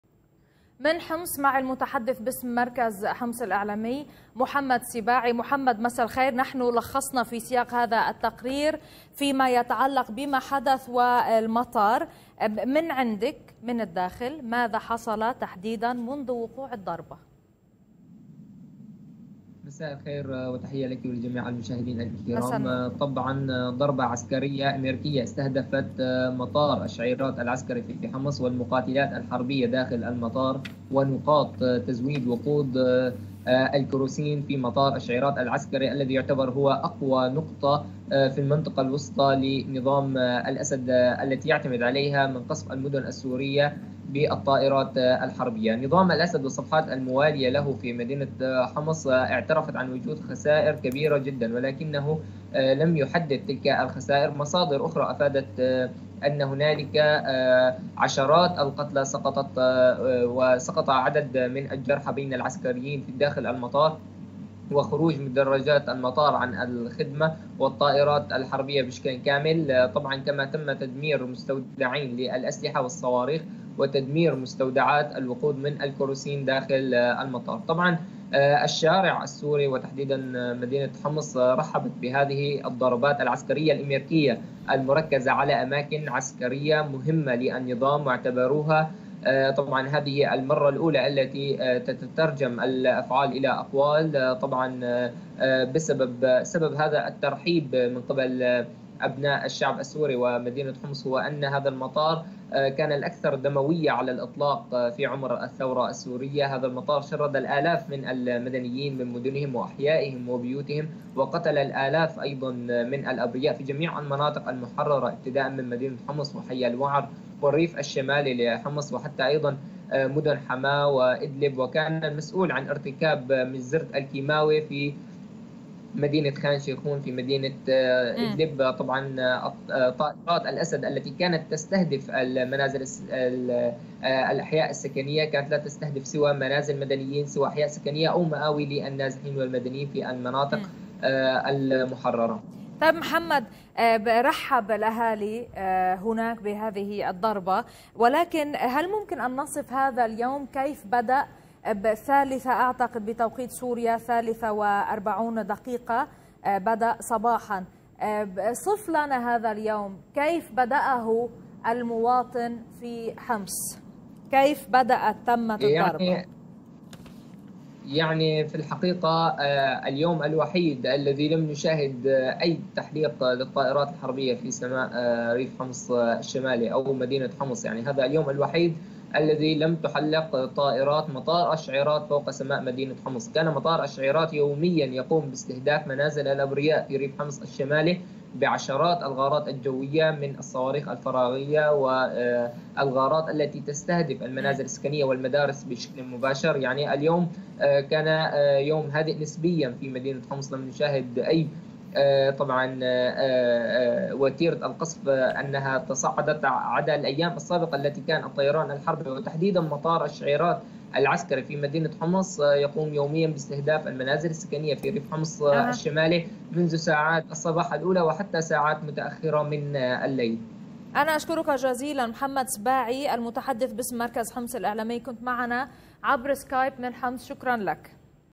أخبار عربية | عشرات الصواريخ الأمريكية تستهدف مواقع عسكرية لنظام
شاهد آيضا:
قيادي بعثي يفضح تاريخ آل الأسد!؟
أكدت مصادر أمريكية ان واشنطن اطلقت نحو 70 صاروخا موجها عالي الدقة على أهداف في سوريا ضمت قاعدة وجوية وتحدثت مصادر عن استهداف موقع للاسلحة الكيميائية، وذلك بصواريخ من نوع توماهوك عابرة للقارات على القاعدة الجوية السورية في حمص تمت بالفعل.